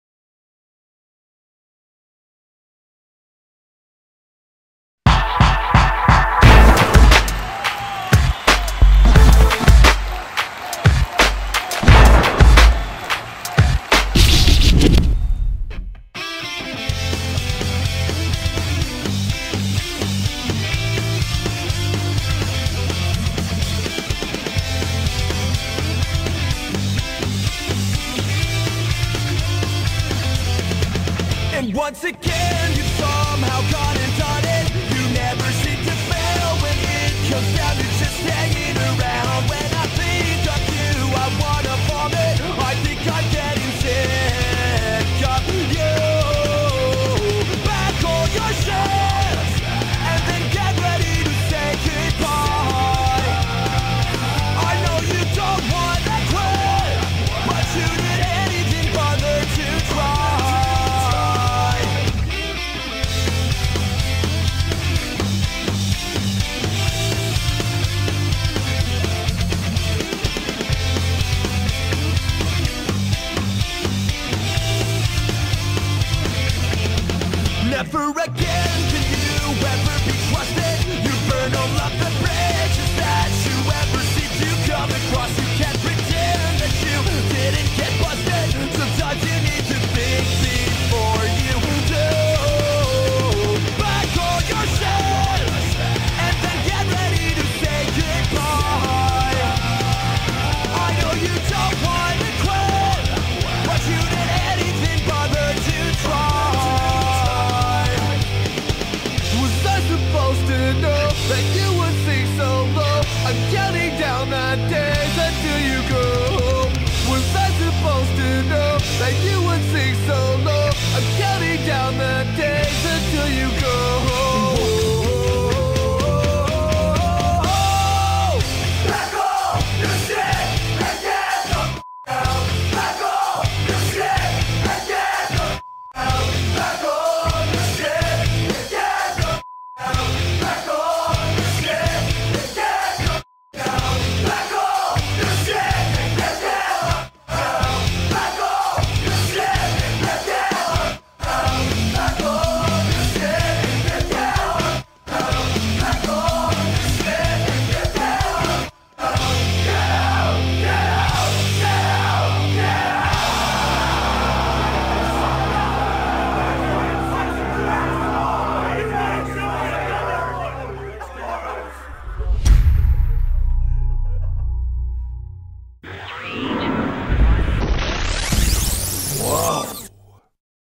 Stars Of the Richmond Cup 2K11
After one of the toughest seasons its no surprise that the winning teams are eager to pack all their gear and get to Lake Land Florida. Spectators and online communities are already calling this "the most evenly matched Richmond cup yet". It's going to come down to who shows up to play.